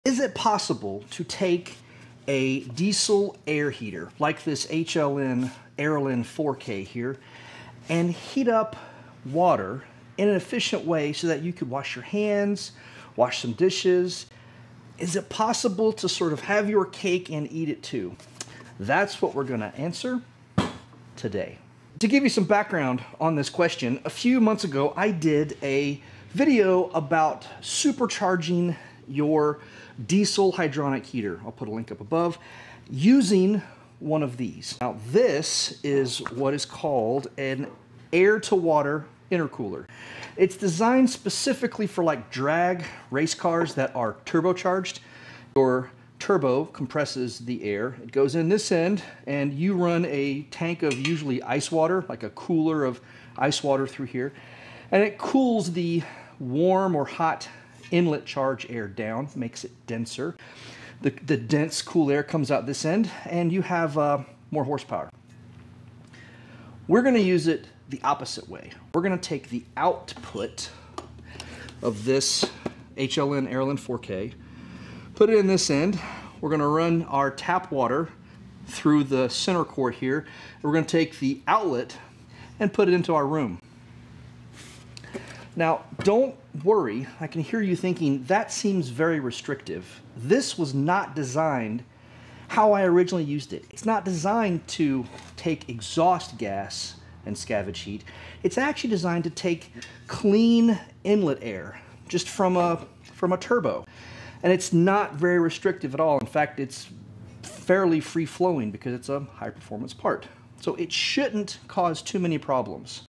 Diesel Heater Water Heating? Efficient DIY Explained!
Can a diesel air heater efficiently heat water for washing? We explore using an air-to-water intercooler—typically for turbo cars—in reverse to scavenge heat. Can we have our cake and eat it too?